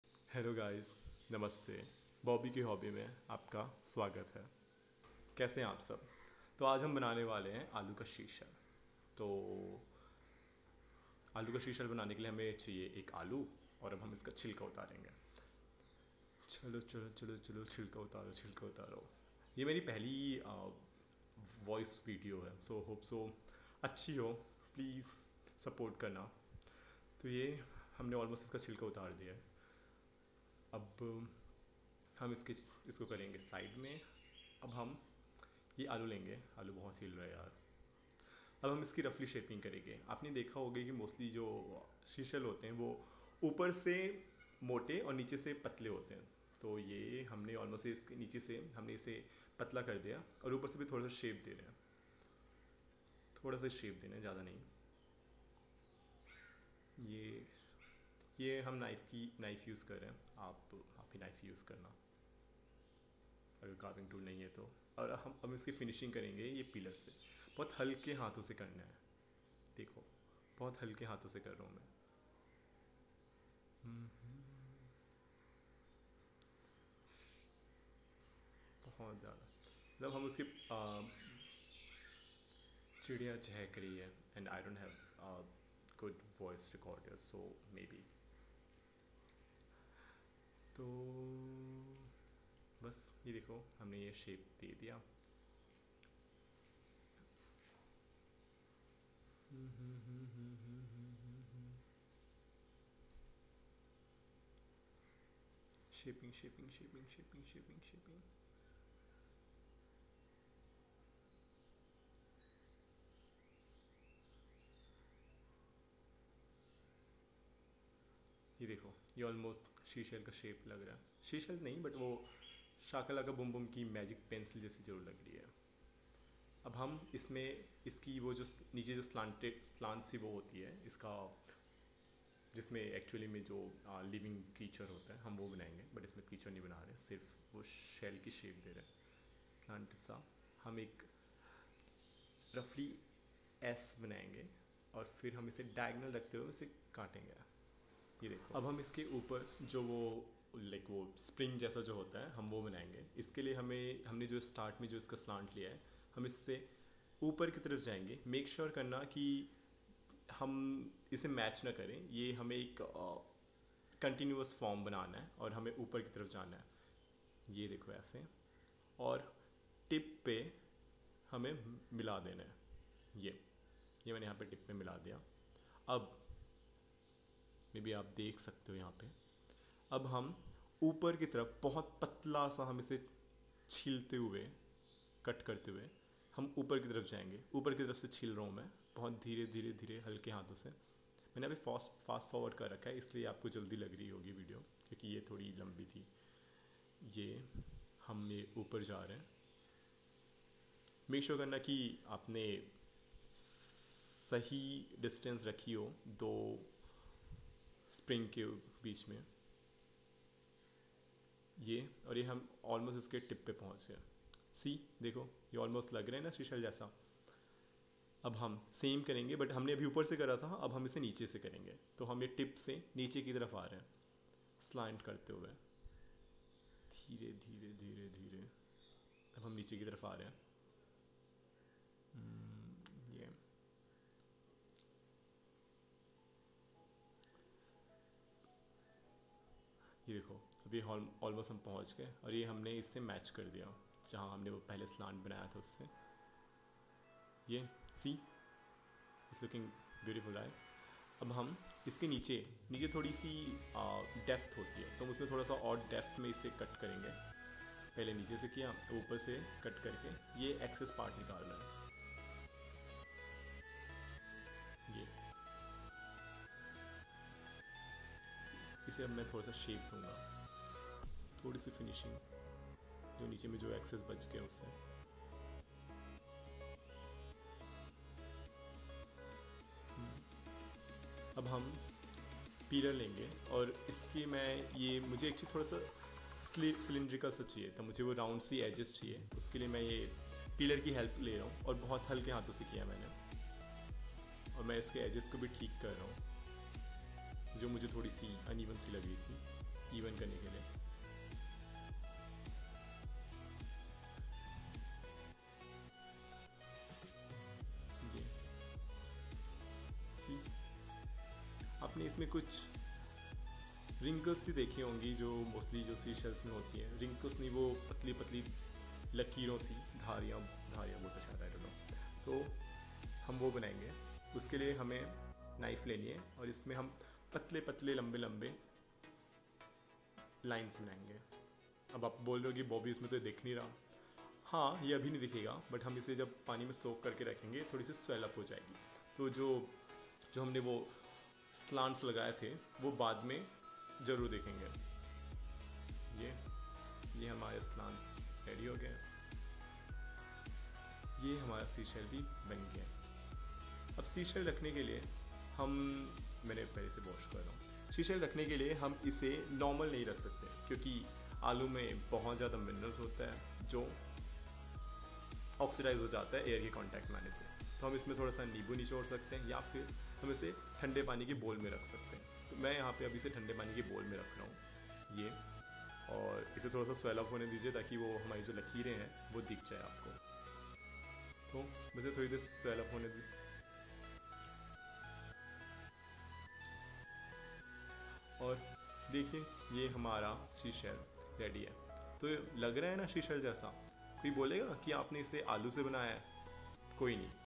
Potato carving, aaloo se banae seashell.
Aaloo se bnao seashell, koi believe hi nahi karega ki ye aapne banaya hai.